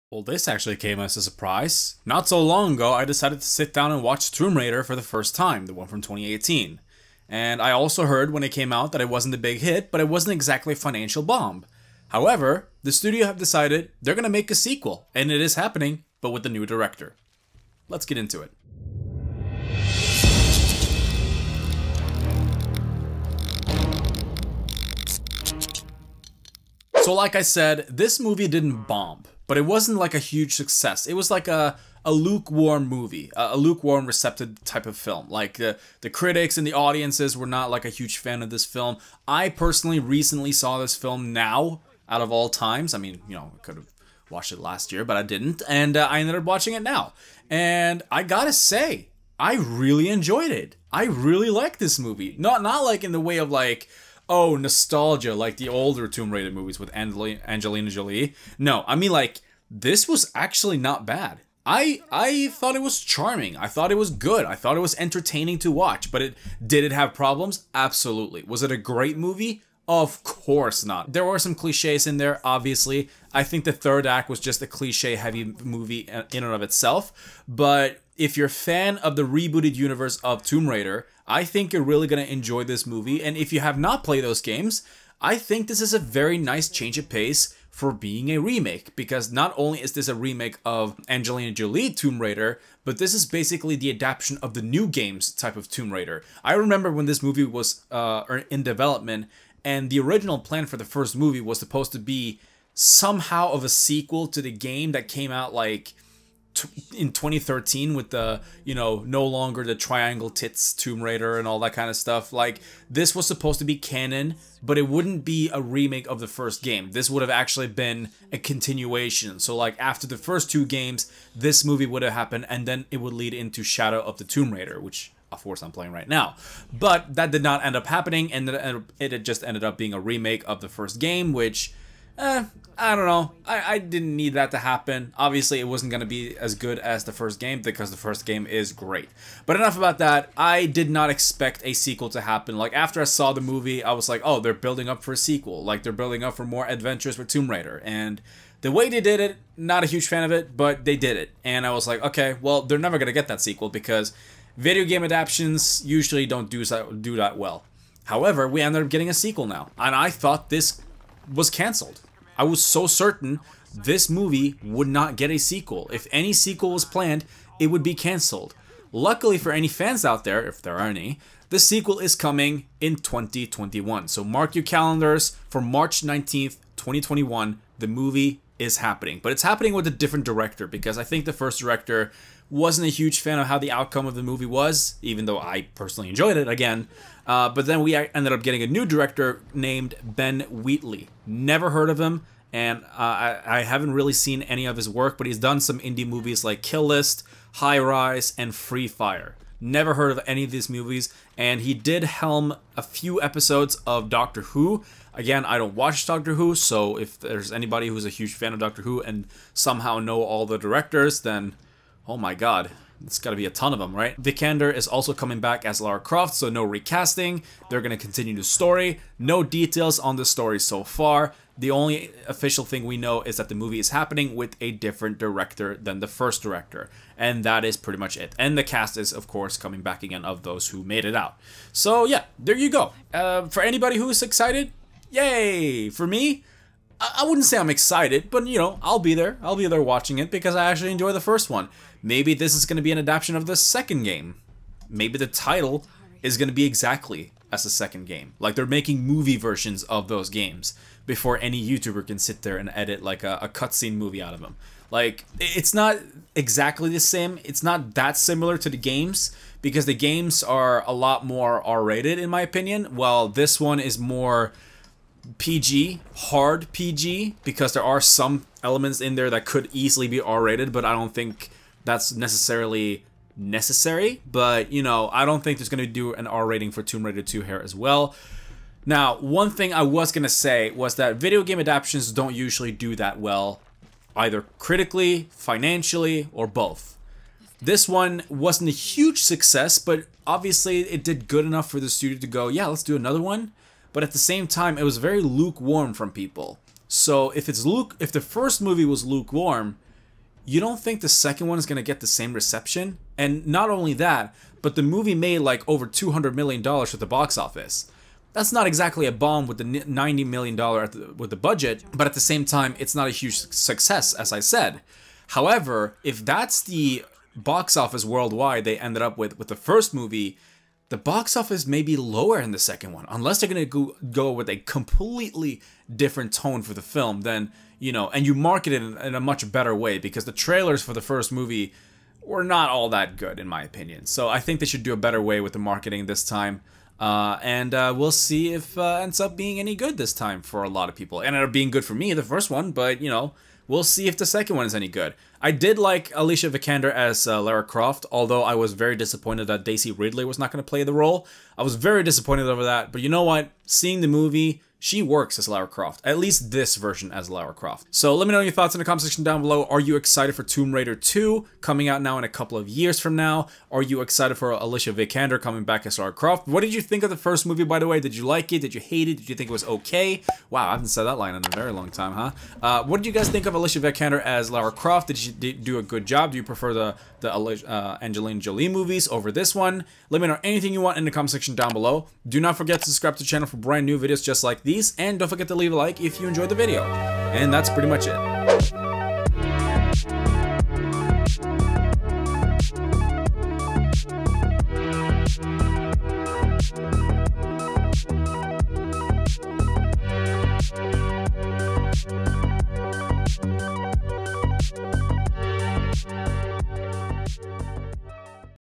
Tomb Raider 2 Confirmed For 2021 - Alicia Vikander Returns
OPEN ME

Tomb Raider 2 is officially a go. MGM has set a release date for the sequel and has firmed up the creative team that will bring the iconic video game character back to the big screen. Alicia Vikander, who portrayed Lara Croft in the 2018 movie, is returning to reprise her role. Plot details are currently being kept under wraps, but the indication is that the studio may be heading in a different direction with this one, given who they've brought in behind the camera.

Are you excited for the upcoming Tomb Raider sequel starring Alicia Vikander once again?